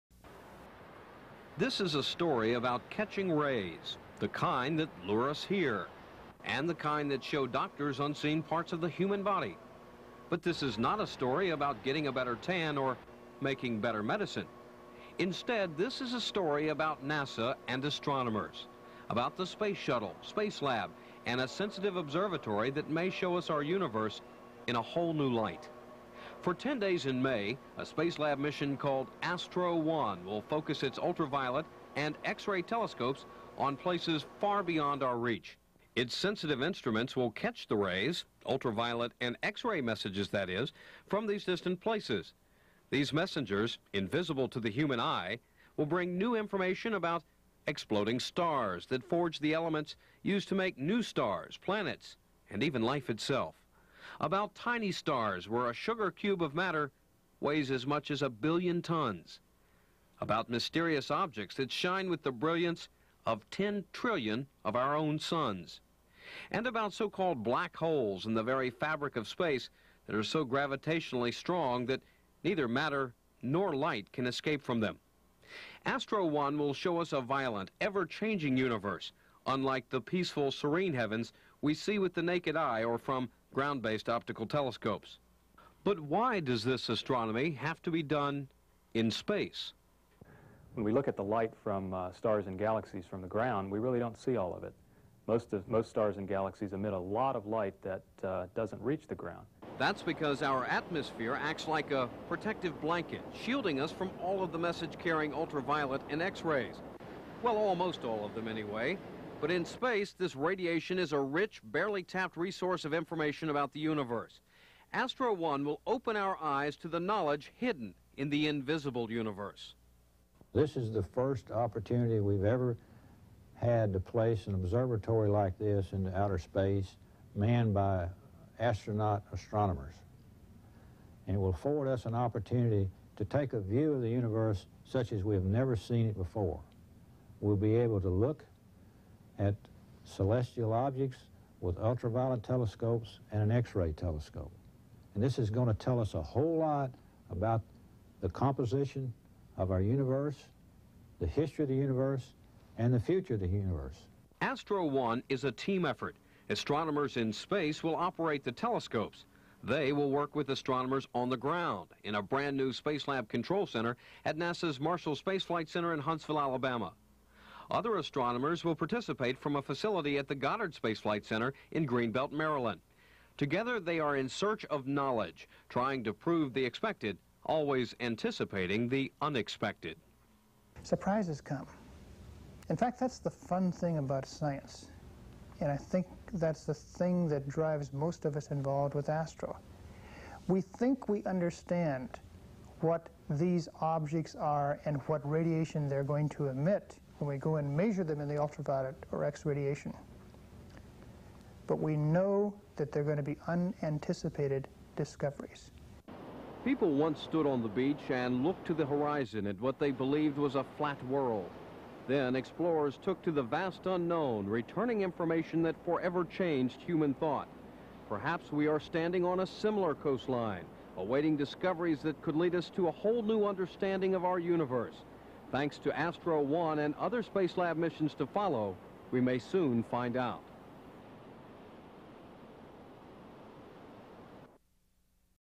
ASTRO-1 to Explore Invisible Universe
This video explains the ASTRO 1 observatory and its ten day mission aboard SpaceLab on NASA's Space Shuttle, which Marshall Space Flight Center (MSFC) and Goddard Space Flight Center (GSFC) astronomers will use to study distant stars, supernovae, and black holes. The observatory contains ultraviolet and x ray telescopes that will capture images earth-bound observatories can't, due to interference from the earth's atmosphere. The video contains footage of the instrument being loaded on the shuttle, animations of anticipated images to be captured, and scenes of the SpaceLab Control Center at MSFC.